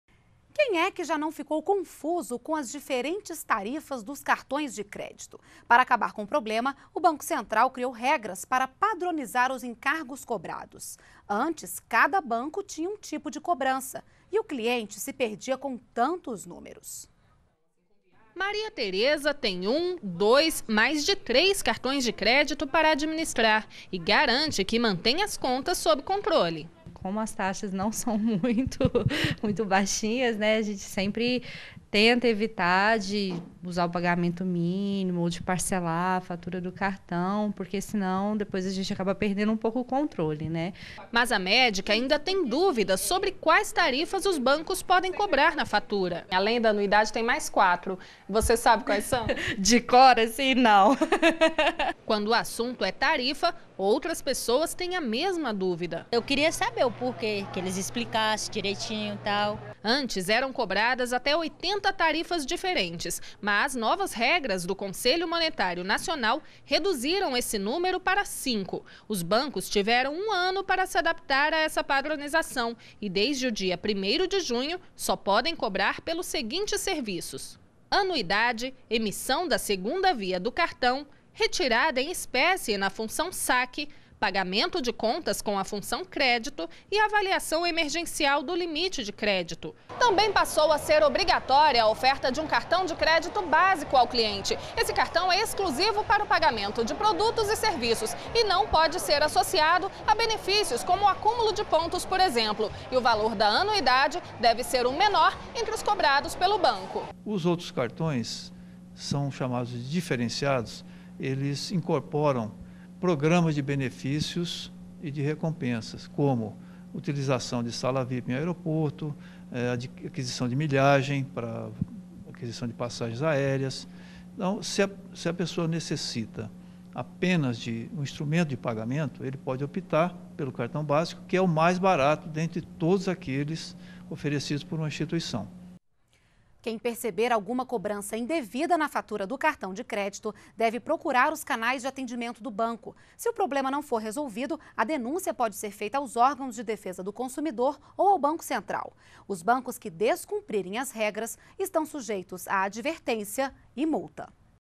Banco Central cria regras para cobrança de tarifas de cartões de crédito
NBR NOTÍCIAS - 09.06.12:   O Banco Central criou  regras para padronizar os encargos cobrados dos cartões de crédito . Anteriomente cada banco tinha um tipo de cobrança, confudindo o cliente com vários números de tarifas. A partir do dia 1° de julho, todos as instituições bancárias vão ter que cumprir as regras estabelecidas. Qualquer cobrança indevida na fatura do cartão de crédito, os cidadãos devem procurar os canais de atendimentos do banco. Se o problema não for resolvido, a denúncia pode ser feita aos órgãos de defesa do consumidor ou ao Banco Central. Os bancos que descumprirem as regras estão sujeitos à advertência e multa.